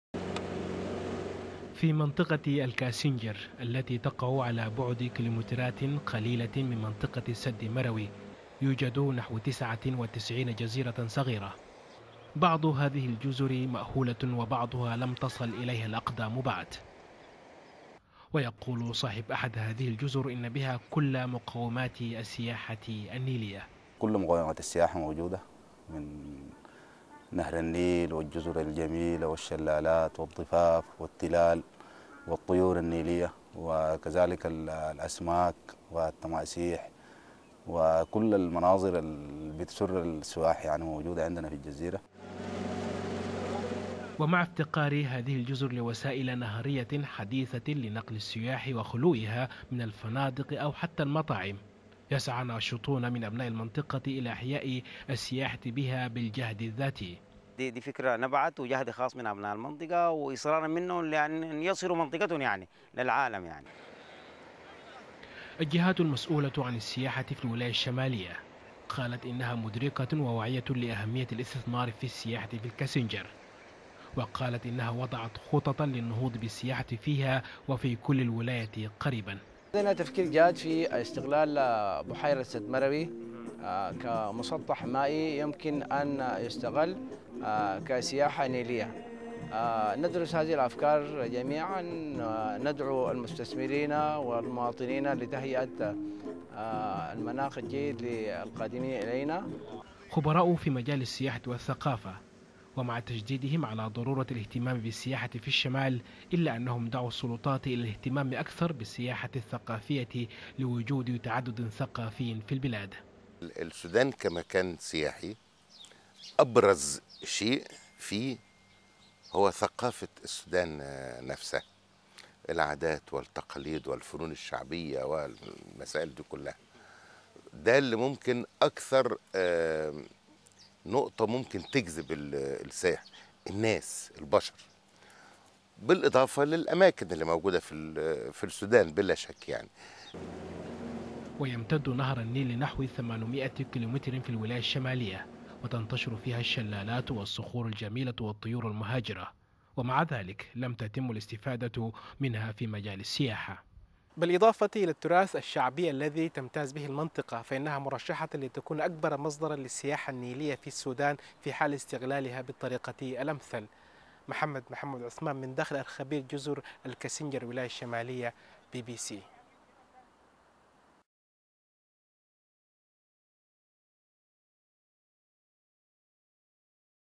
جزر رملية خلابة في قلب النيل في الكاسنجر بالسودان
عشرات الجزر الرملية تنتشر في نهر النيل بمنطقة الكاسنجر بالولاية الشمالية حيث كل مقومات السياحة في النيل، غير أنه لم يتم استغلالها حتى الأن لجذب السياح اليها.